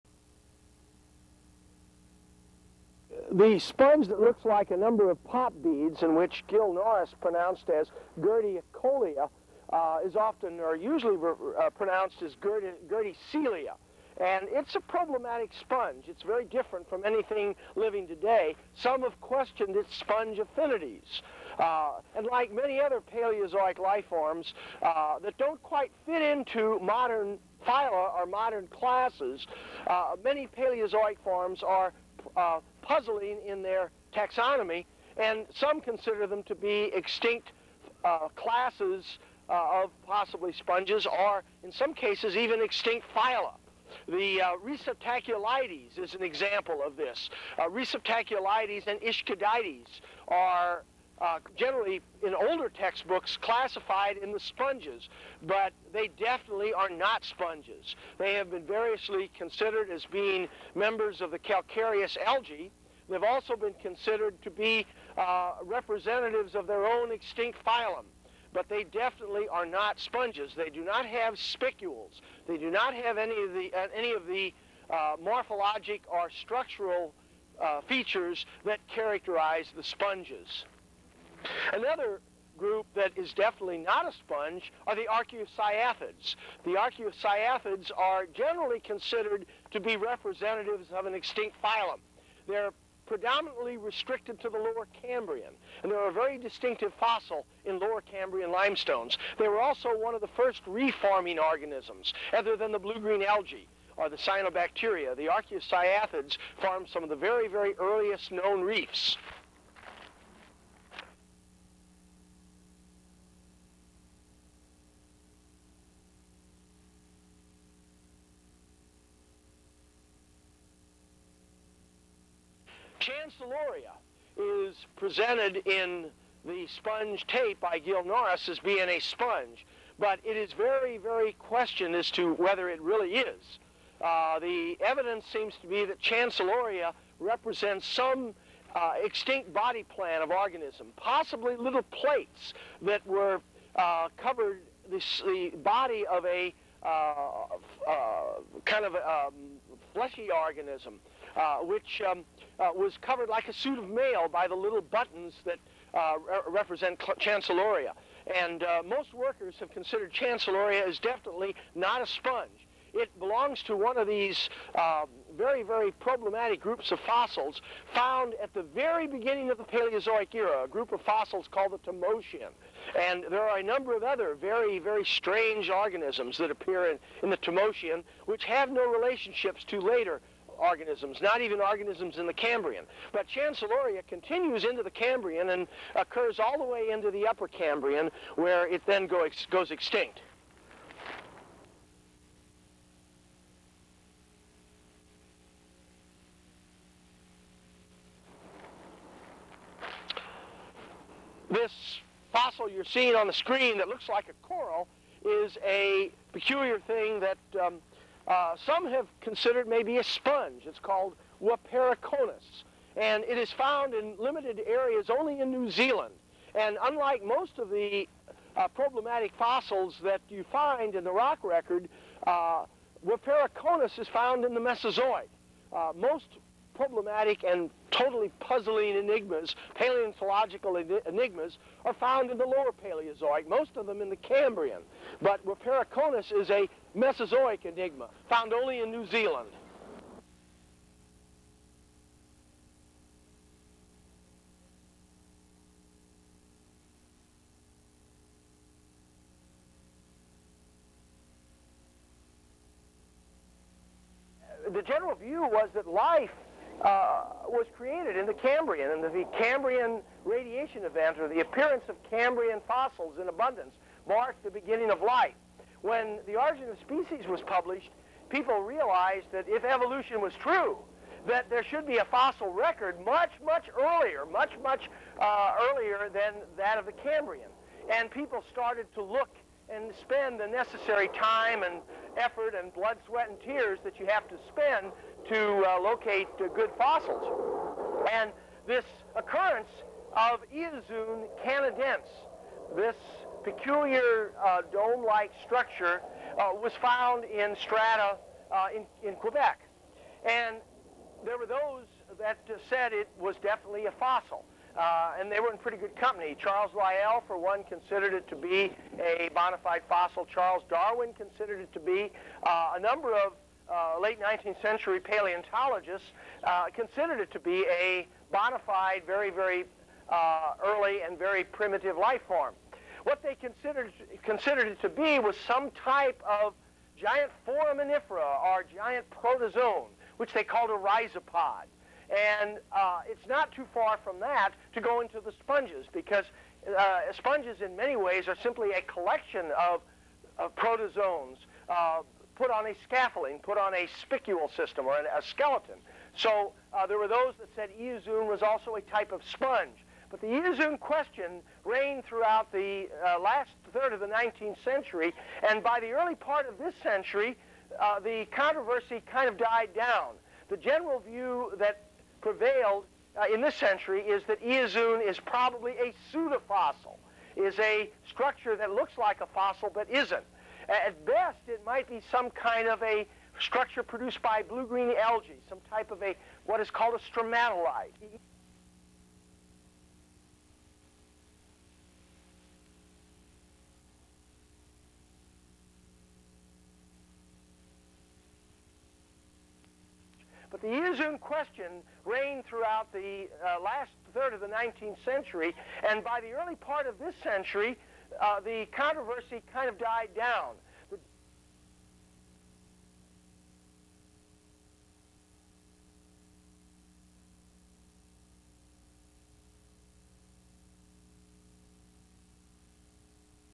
Fossil sponges, Eozoon canadense
A variety of problematic fossils are presented all of which one time or another were considered to be sponges. Paleontologists of the 19th and early 20th century were especially interested in sponges as they were the most primitive animals known at that time. Receptaculites was especially of interest as it is a common and conspicious  fossil in relatively ancient strata (Ordovician) of mid continent North America. The problematic archeocyathids and Chancelloria, both found in Cambrian age rocks were also considered as being sponges. The pseudofossil eozoon canadense was also fasvorably compared to sponges before it was determined to be a pseudofossil.